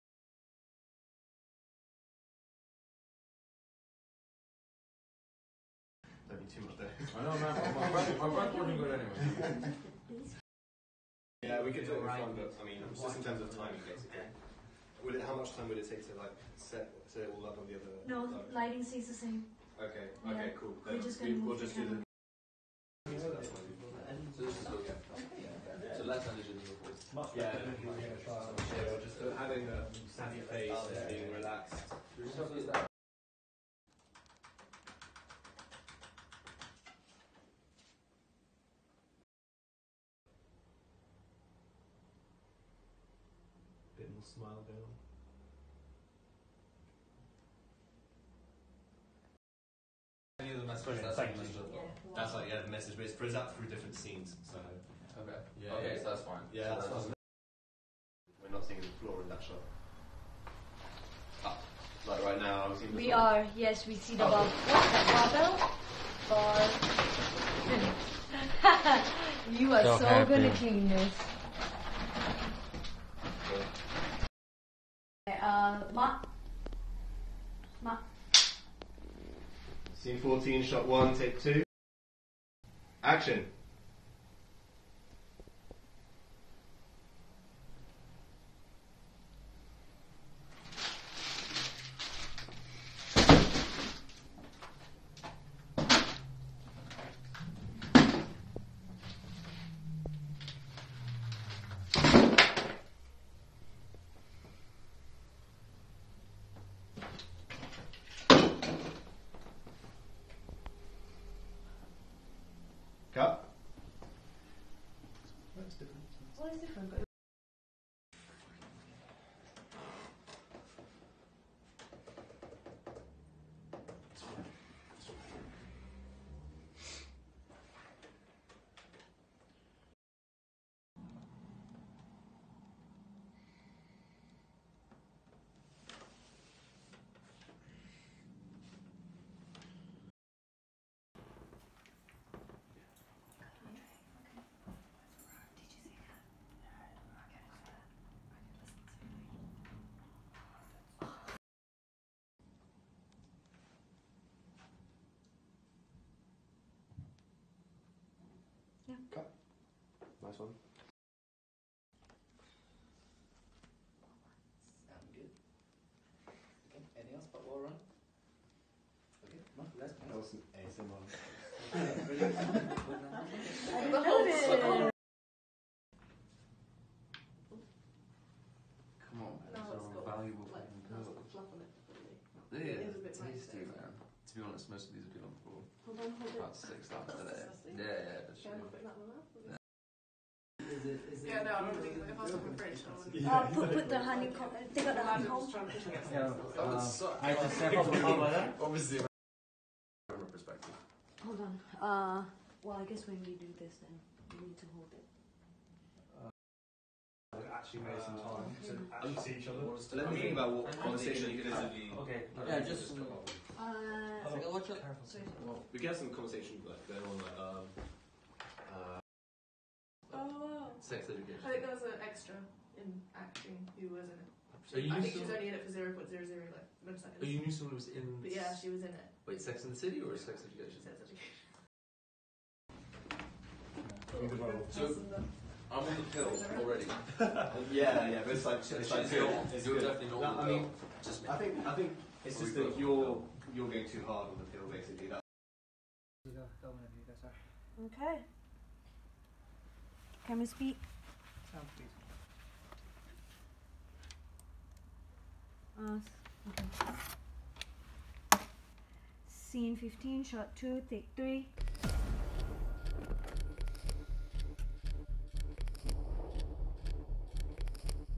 Behind the scenes : Harder, Better, Faster, Stranger (2019)
Behind the scenes of "Harder, Better, Faster, Stranger" shot in Bournemouth (UK) between the 12th November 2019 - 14th November 2019.